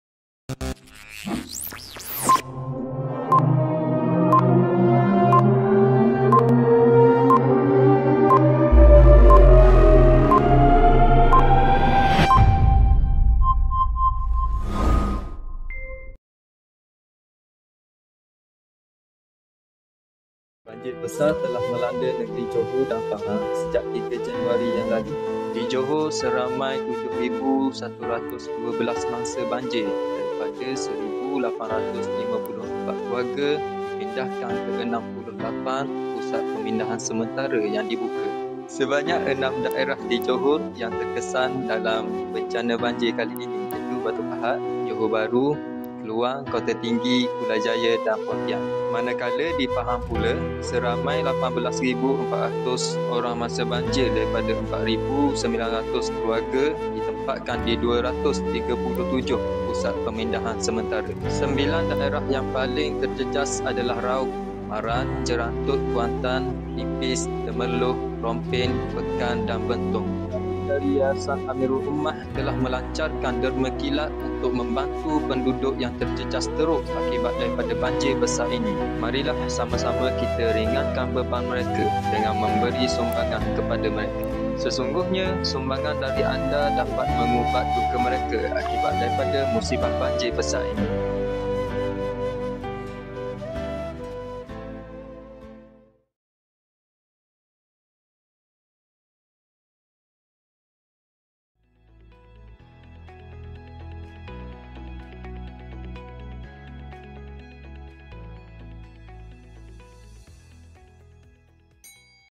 "Hanya Nampak ATAP SAHAJA!!"...
"Bersabung Nyawa"

RECAP : Bencana banjir besar yang melanda Johor dan Pahang pada awal Januari 2022 yang lalu telah menyebabkan banyak kemusnahan dan kehilangan tempat tinggal bahkan meranapkan segala yang ada di sekitarnya.

Di Johor seramai 7112 daripada jumlah mangsa banjir terdiri daripada 1854 keluarga dipindahkan ke 68 Pusat Pemindahan Sementara (PPS). Banjir tersebut melibatkan 6 daerah yang terkesan. Manakala di Pahang,  seramai 18400 orang mangsa banjir terdiri daripada 4900 keluarga ditempatkan di 237 Pusat Pemindahan Sementara (PPS) yang telah dibuka melibatkan 9 daerah yang terjejas.

Yayasan Ammirul Ummah telah melancarkan Derma Kilat bagi membantu mangsa banjir yang terjejas teruk akibat daripada banjir besar yang melanda. Hasil daripada kutipan derma kilat tersebut, pihak Yayasan Ammirul Ummah telah berjaya menyumbang sejumlah 300 Kit Solat Ummah kepada sebahagian mangsa yang terjejas.

Kami mengucapkan jutaan terima kasih kepada semua penyumbang yang telah menginfakkan secebis daripada harta mereka untuk membantu mereka yang memerlukan di luar sana. Semoga Allah SWT mempermudahkan segala urusan anda semua dan segala usaha murni kita diberkati olehNya.